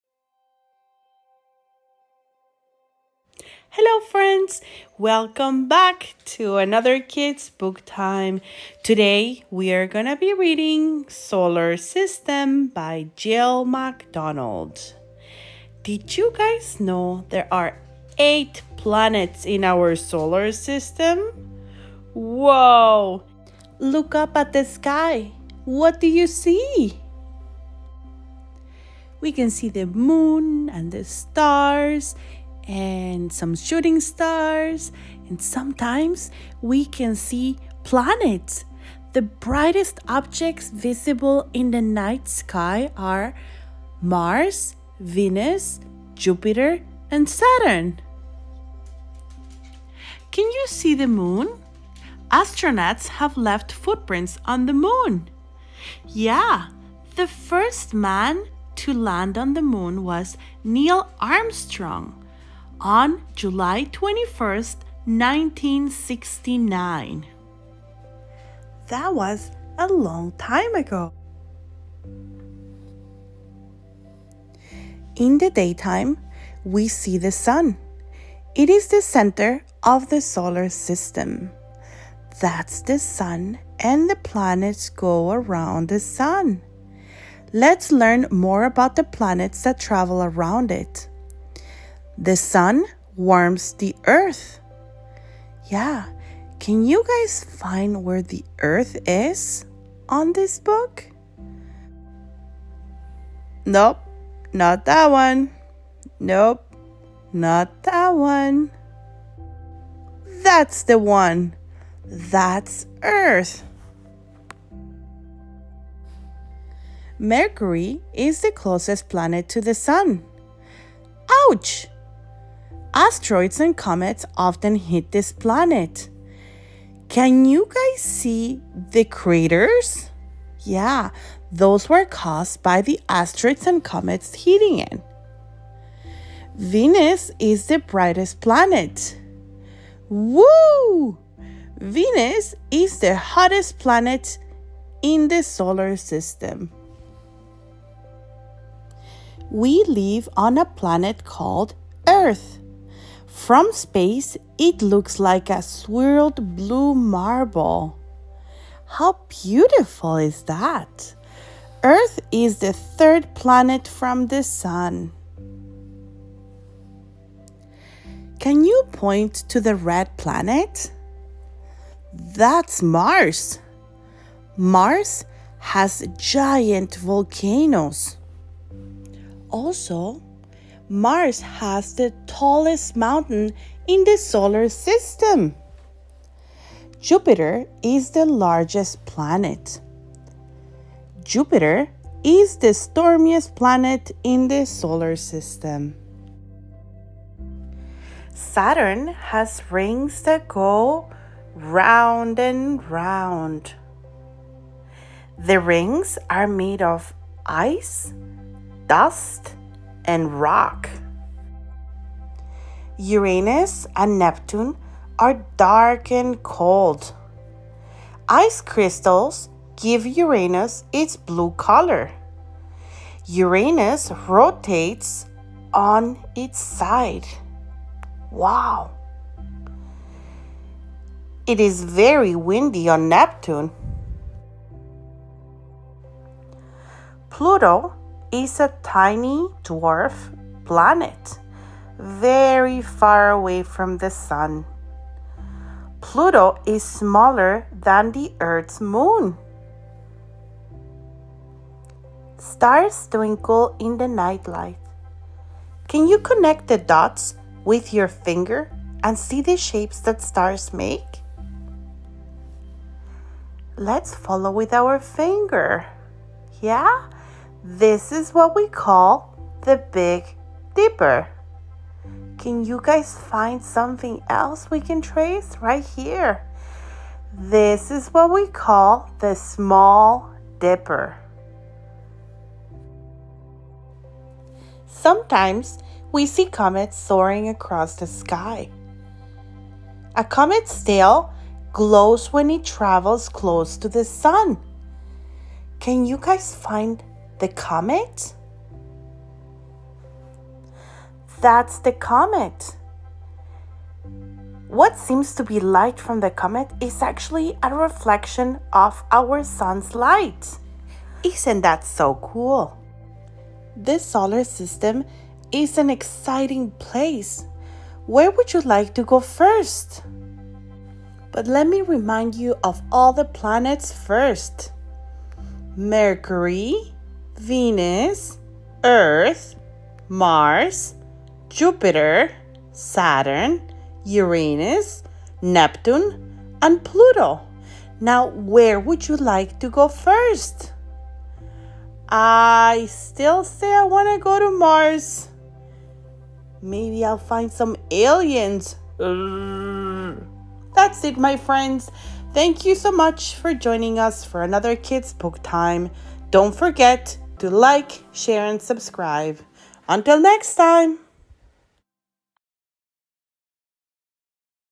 🪐 Solar System 🌞
Hello Friends! 👋 Welcome to @Kidsbooktime

Today, we will be reading:

By Jill McDonald

Look up in the sky! Can you see the moon? What else is there?
Babies and toddlers can discover the world around
them with Hello, World!, designed to share simple concepts about science, nature, and culture.